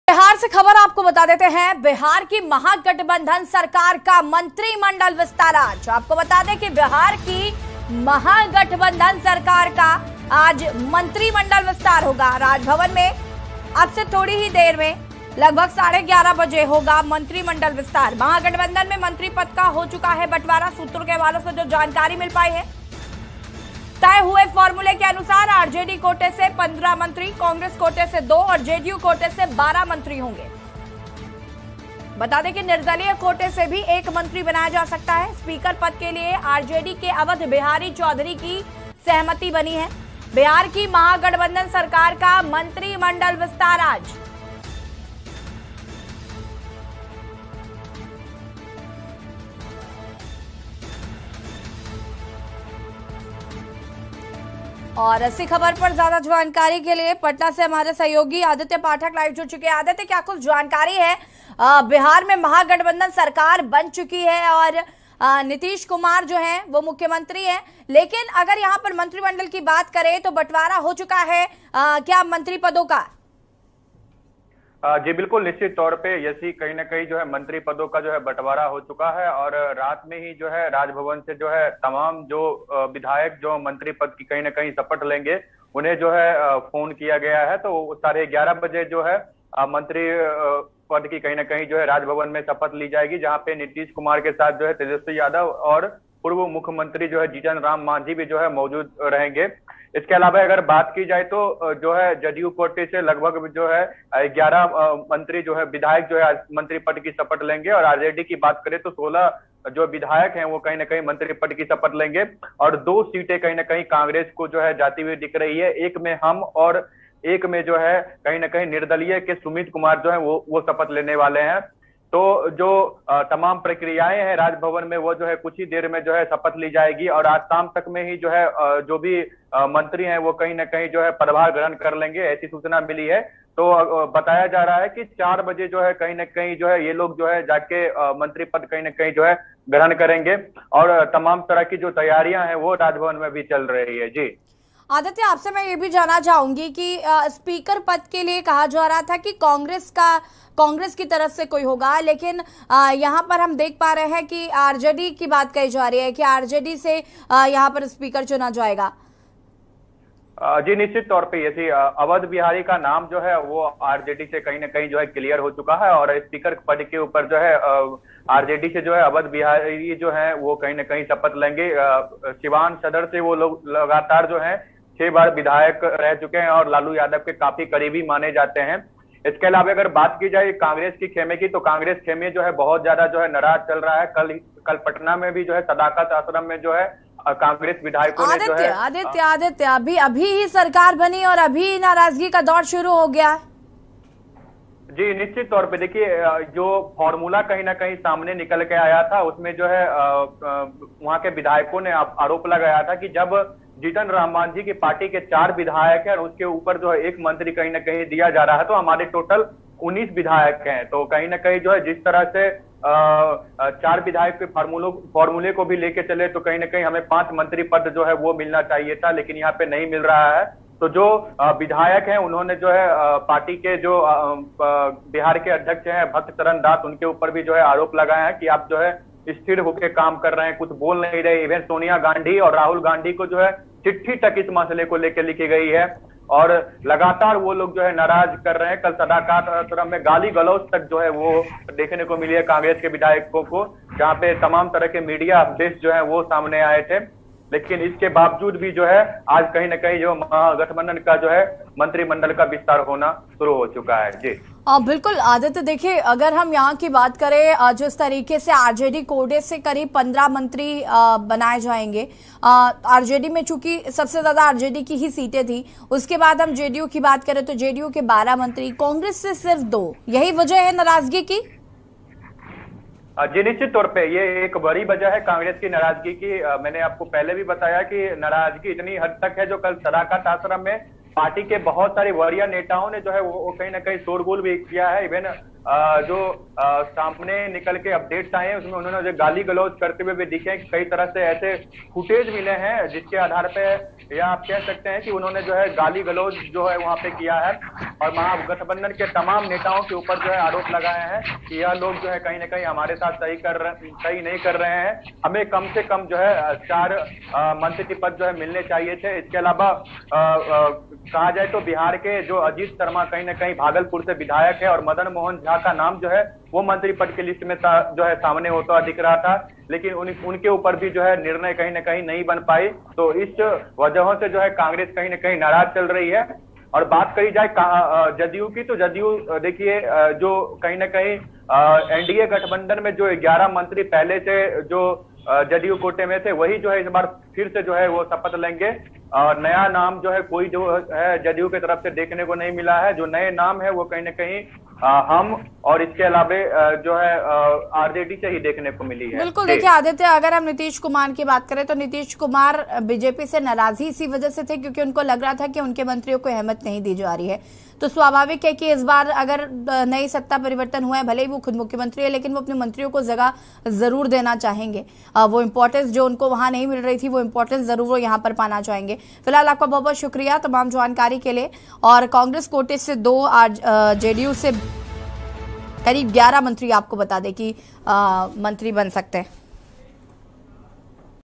Patna : बिहार की महागठबंधन सरकार का मंत्रिमंडल विस्तार आज || K News India
बिहार की महागठबंधन सरकार का मंत्रिमंडल विस्तार आज. राजभवन में 11.30 बजे होगा मंत्रिमंडल विस्तार .महागठबंधन में मंत्री पद का हो चुका बंटवारा-सूत्र. तय हुए फार्मूले के अनुसार RJD कोटे से 15 मंत्री.